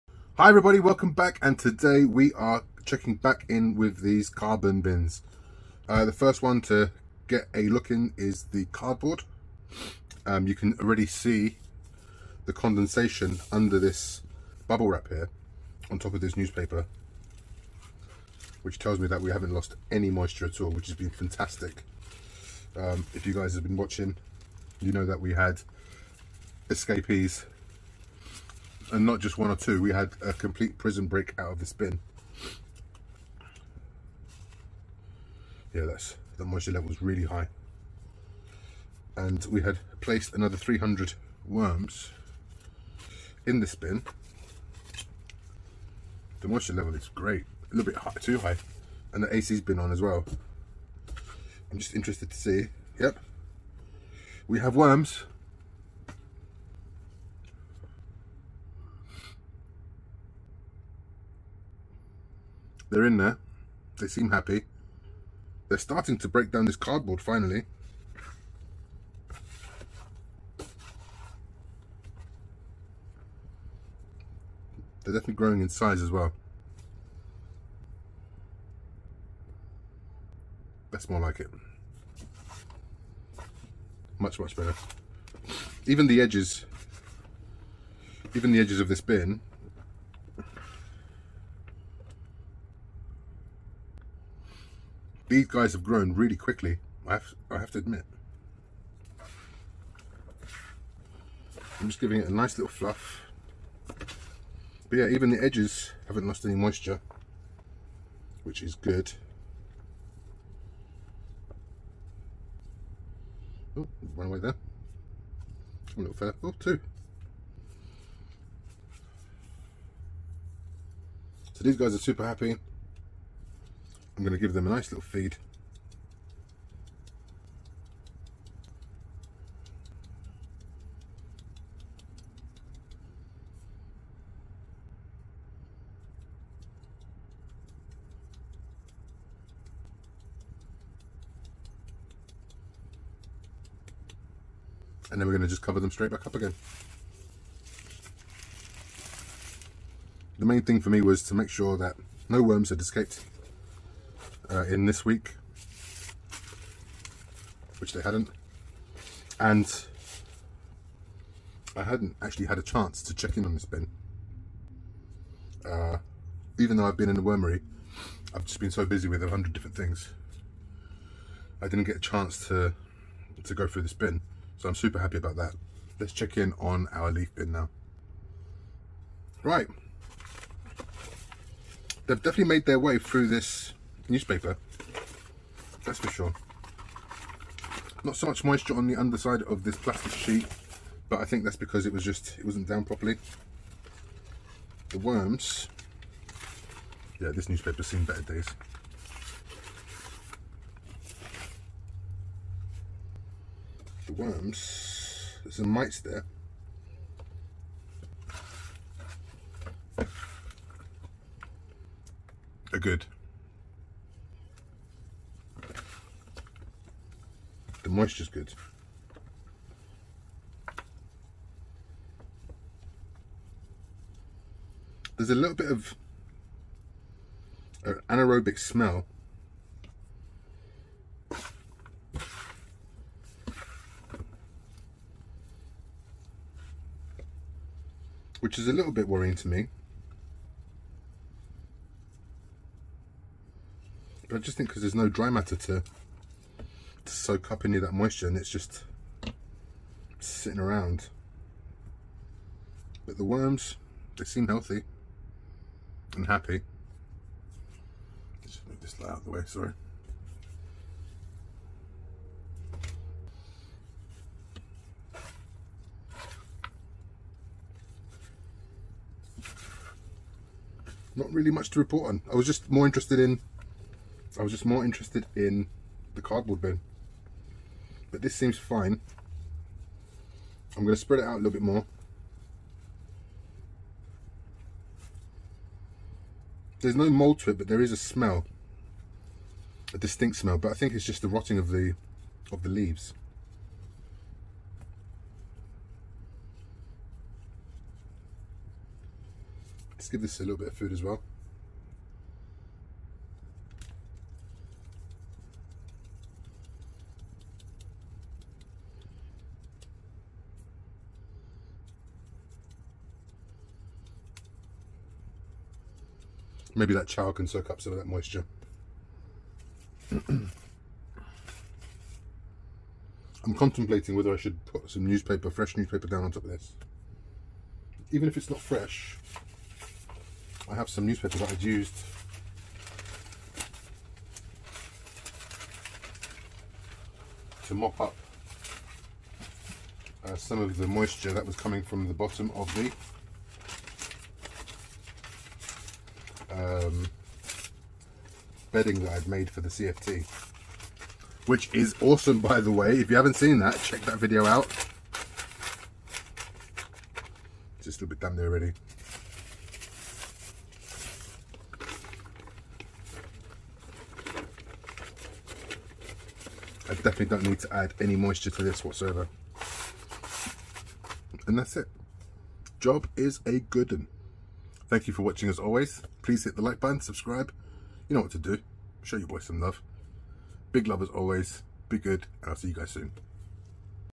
Worm Bin Carbon Experiment Part 4 - Cardboard VS Leaves - African Night Crawlers
I decided to limit the updates to monthly videos but show what happens in that time in that monthly video. Anyway, there was a bit of a hiccup. I got Bronchitis but I've been dying to tell you what happened with one of the bins!

Ever wondered what Carbon source produces better Worm Castings? Me too! I've been planning this experiment for some time now and with the delivery of my new tubs, the day has finally come to get it going. I've watched many a video where other Worm farmers speculate on a number of outcomes but I'm yet to see a side by side with a certified soil test at the end of it.